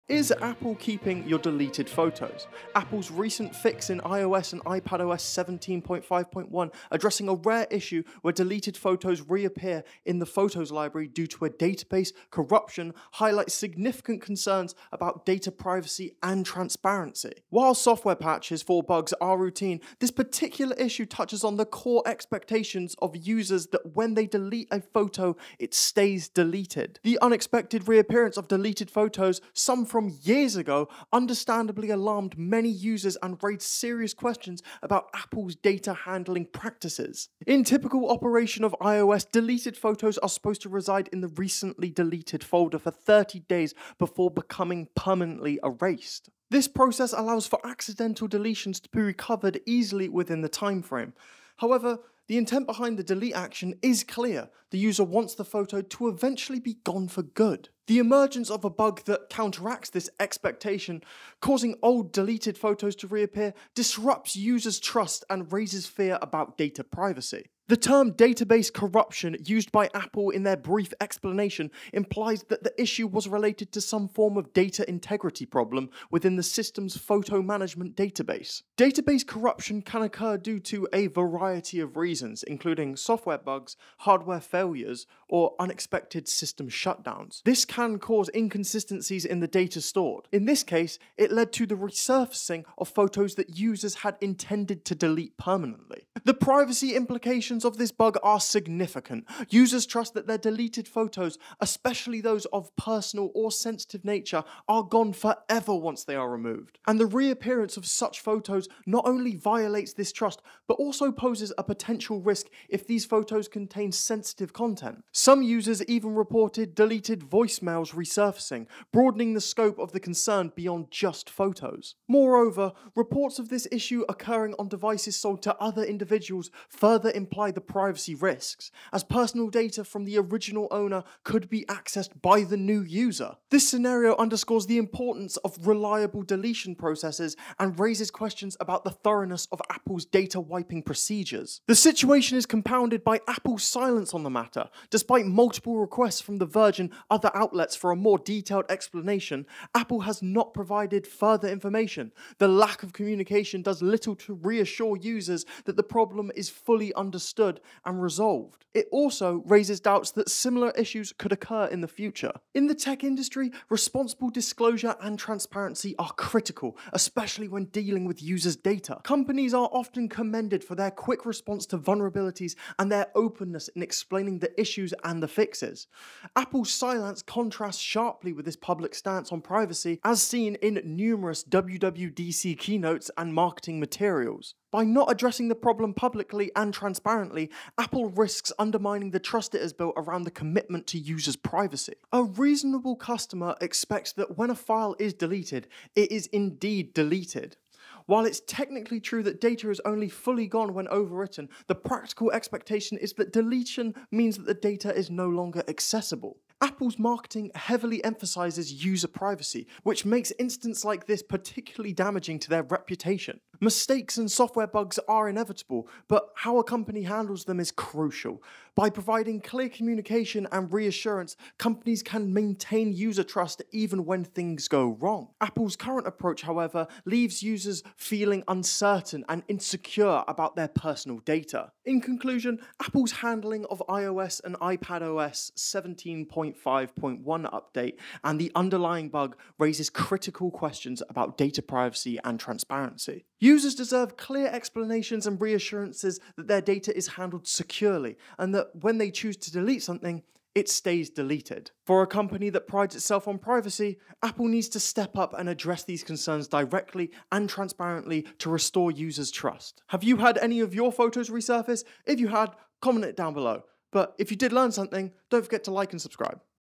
Apple HAS your DELETED files!
Apple's recent fix in iOS and iPadOS 17.5.1, addressing a rare issue where deleted photos reappeared in the Photos library due to database corruption, highlights significant concerns about data privacy and transparency.

While software patches for bugs are routine, this particular issue touches on the core expectation of users that when they delete a photo, it stays deleted.

The unexpected reappearance of deleted photos, some from years ago, understandably alarmed many users and raised serious questions about Apple's data handling practices.

In the typical operation of iOS, deleted photos are supposed to reside in the Recently Deleted folder for 30 days before being permanently erased.

This process allows for accidental deletions to be recovered easily within that time frame. However, the intent behind the delete action is clear: the user wants the photo to eventually be gone for good.

The emergence of a bug that counteracts this expectation—causing old, deleted photos to reappear—disrupts user trust and raises fears about data privacy.

The term "database corruption" used by Apple in their brief explanation implies that the issue was related to some form of data integrity problem within the system's photo management database.

Database corruption can occur due to a variety of reasons, including software bugs, hardware failures, or unexpected system shutdowns, which can cause inconsistencies in the data stored. In this case, it led to the resurfacing of photos that users had intended to delete permanently.

The privacy implications of this bug are significant. Users trust that their deleted photos, especially those of a personal or sensitive nature, are gone forever once they are removed.

The reappearance of such photos not only violates this trust but also poses potential risks if these photos contain sensitive content. Some users reported that even deleted voicemails were resurfacing, broadening the scope of the concern beyond just photos.

Moreover, reports of this issue occurring on devices sold to other individuals further amplify the privacy risks, as personal data from the original owner could be accessed by the new user.

This scenario underscores the importance of a reliable deletion process and raises questions about the thoroughness of Apple's data wiping procedures.

The situation is compounded by Apple's silence on the matter. Despite multiple requests from The Verge and other outlets for a more detailed explanation, Apple has not provided further information. This lack of communication does little to reassure users that the problem is fully understood and resolved. It also raises doubts about whether similar issues could occur in the future.

In the tech industry, responsible disclosure and transparency are critical, especially when dealing with user data.

Companies are often commended for their quick response to vulnerabilities and their openness in explaining the issues and the fixes. Apple's silence contrasts sharply with its public stance on privacy and security, as seen in numerous WWDC keynotes and marketing materials. By not addressing the problem publicly and transparently, Apple risks undermining the trust it has built around its commitment to user privacy.

A reasonable customer expects that when a file is deleted, it is indeed deleted. While it's technically true that data is only fully gone when overwritten, the practical expectation is that deletion means the data is no longer accessible. Apple's marketing heavily emphasizes user privacy, which makes incidents like this particularly damaging to their reputation.

Mistakes and software bugs are inevitable, but how a company handles them is crucial. By providing clear communication and reassurance, companies can maintain user trust even when things go wrong. Apple's current approach, however, leaves many users feeling uncertain and insecure about their personal data.

In conclusion, Apple’s handling of the iOS and iPadOS 17.5.1 update and the underlying bug raises critical questions about data privacy and transparency. Users deserve clear explanations and reassurances that their data is handled securely and that when they choose to delete something, it stays deleted. For a company that prides itself on privacy, Apple needs to step up and address these concerns directly and transparently to restore user trust and confidence.